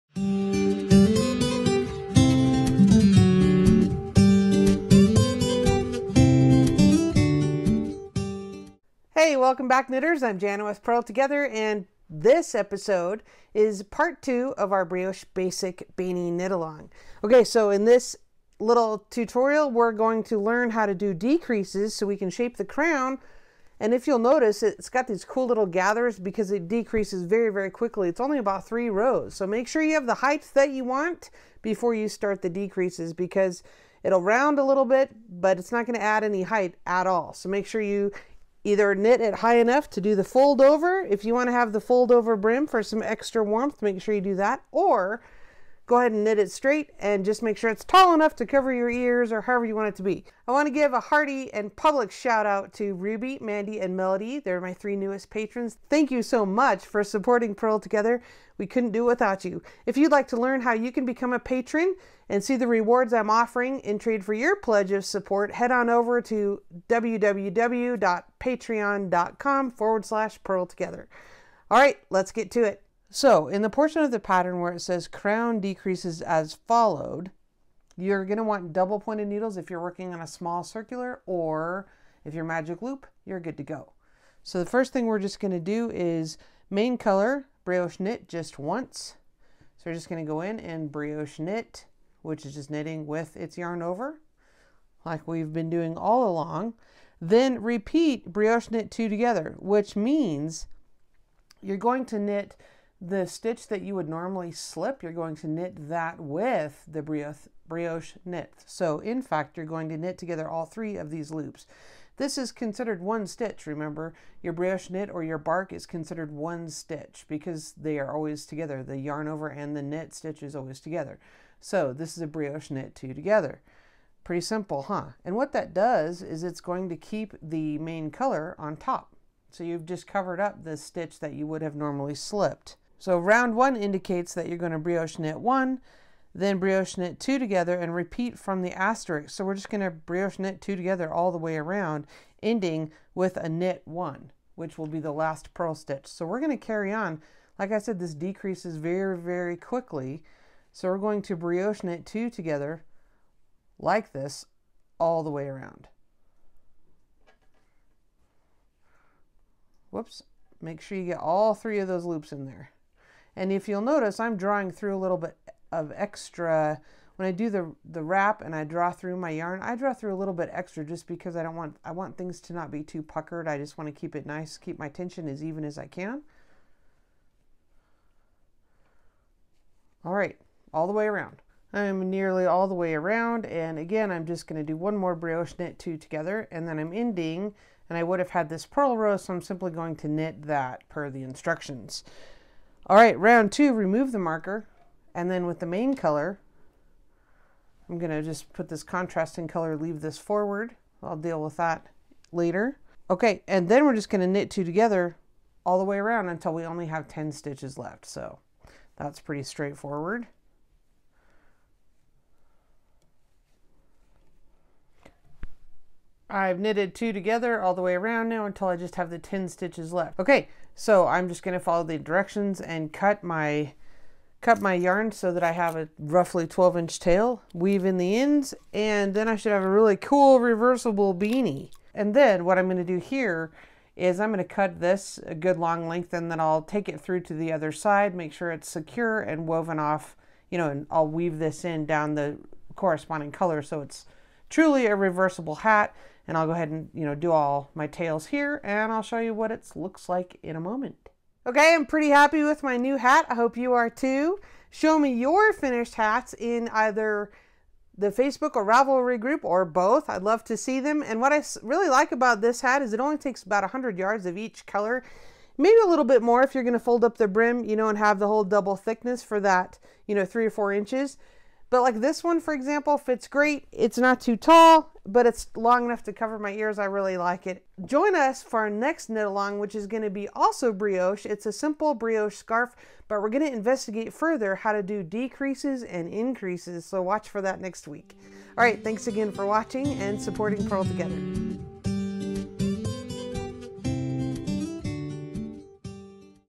Brioche Basic Beanie KAL Part 2
We're finishing up our Brioche Basic Beanie!  Just 3 rounds of decreases and we're done.  Learn how to BrK2tog to decrease the crown of your hat.

My favorite needles are ChaioGoo Complete Interchangeable Set: